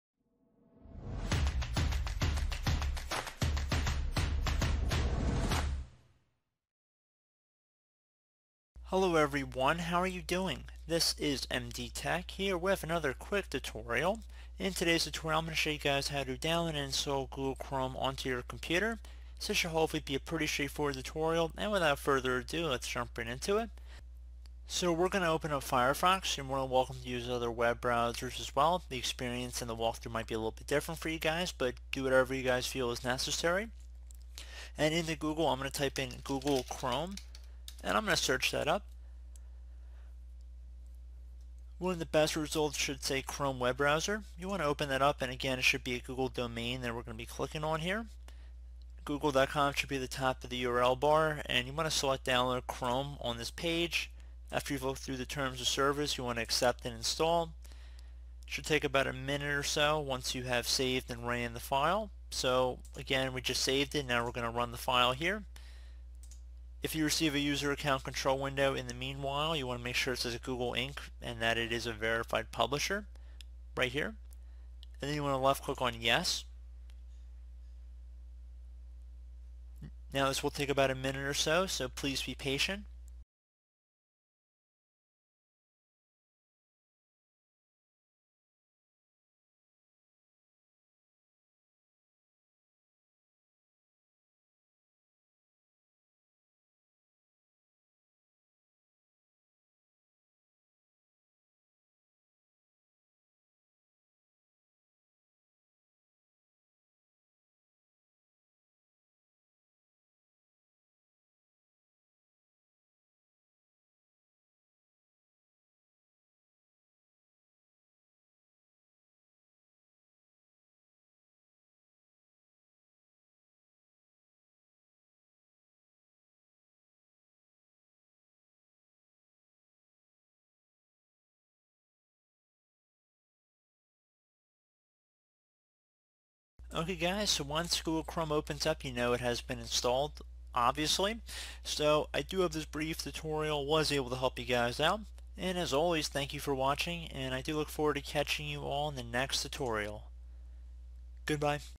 Download Google Chrome Latest Version [Tutorial]
In this tutorial I will  show you how to download and install Google Chrome on Windows.  Download and Install. if you have problem related to Google Chrome will not install in Windows, or you want to get google chrome loaded onto your laptop, computer, or device, this tutorial will be for you.

About Google Chrome (from Wikipedia):

Google Chrome is a freeware web browser developed by Google. It was first released in September 2008, for Microsoft Windows, and was later ported to Linux, macOS, iOS and Android. Google Chrome is also the main component of Chrome OS, where it serves as a platform for running web apps.